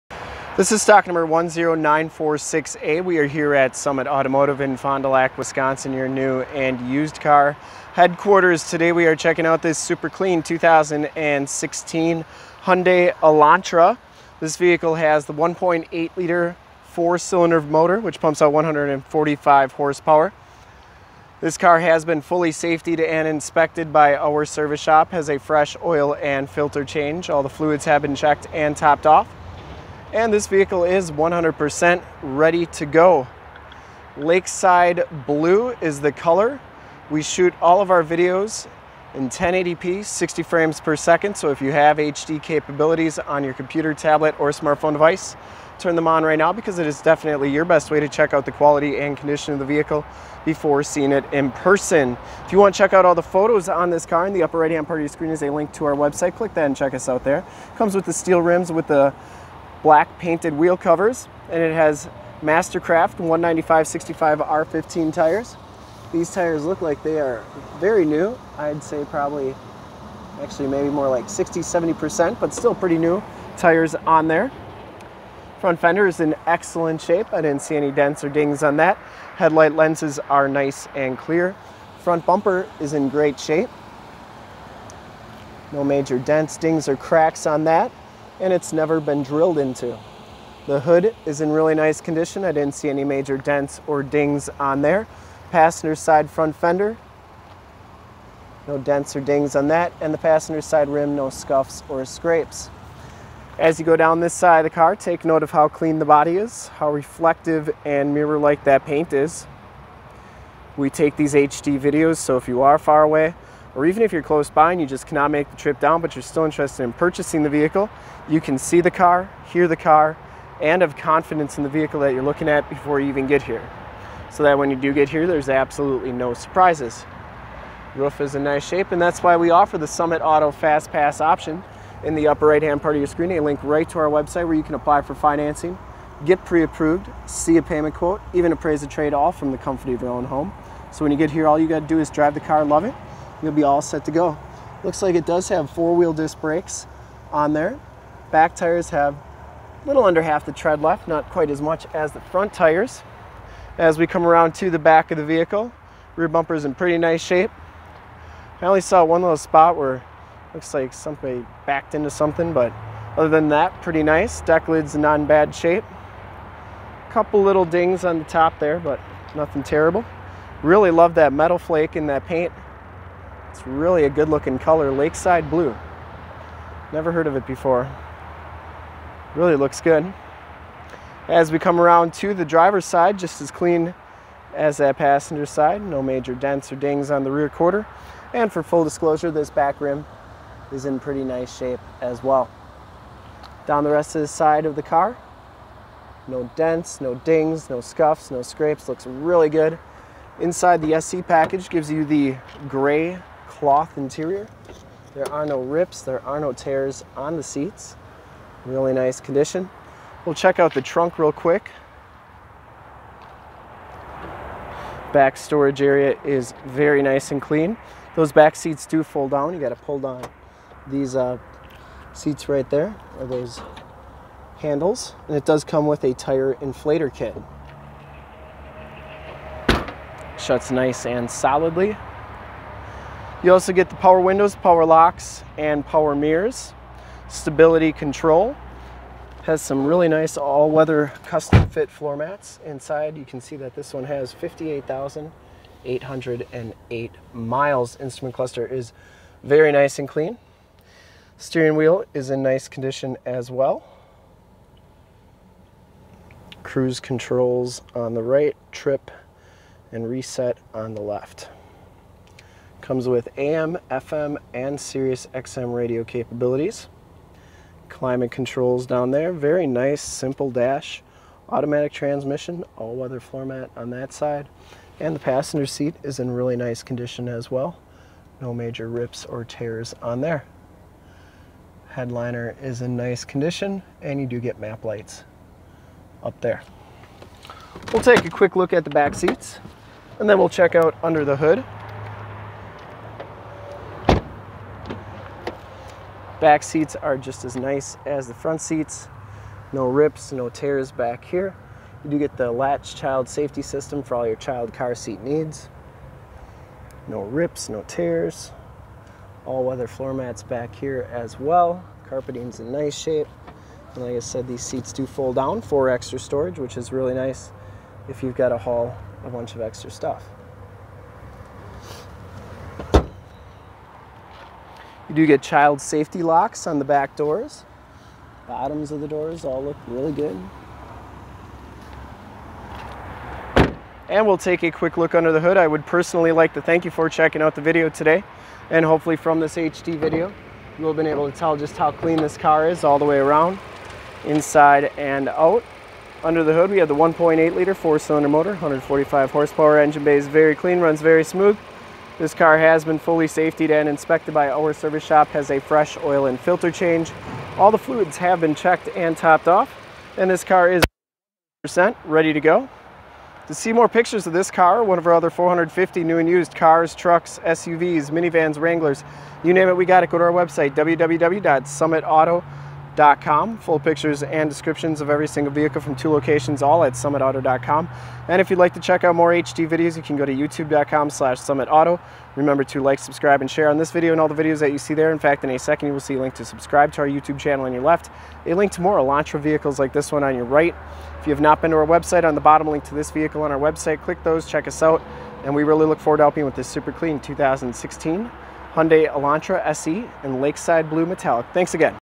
2016 HYUNDAI ELANTRA SE LAKESIDE BLUE METALLIC WALK AROUND REVIEW 10946A SOLD! SUMMITAUTO.com
This 2016 HYUNDAI ELANTRA SE FOUR DOOR IN LAKESIDE BLUE METALLIC is the vehicle we did walk around review of today.

Thank you for checking out this video of this 2016 HYUNDAI ELANTRA SE FOUR DOOR IN LAKESIDE BLUE METALLIC

STOCK: 10946A
PRICE: $9,992
MILES: 58,796
MAKE: HYUNDAI
MODEL: ELANTRA
VIN: 5NPDH4AEXGH786740
LOCATION: FOND DU LAC OSHKOSH WISCONSIN, 54937 TRUCKS ON 41